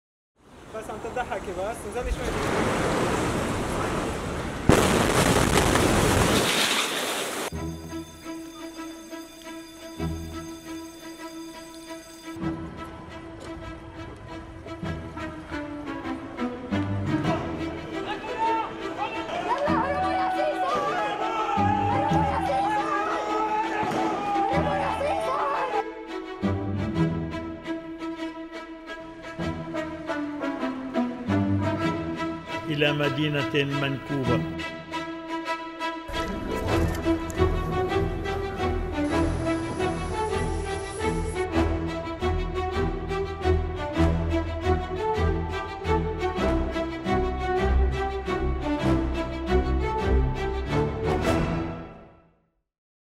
В Бейруте возобновились протесты
Митингующие требуют отставки Президента Ливана Мишеля Ауна и роспуска парламента. Накануне премьер-министр Ливана Хасан Диаб объявил об отставке своего правительства. Что происходит в столице Ливана через неделю после взрыва аммиачной селитры в порту?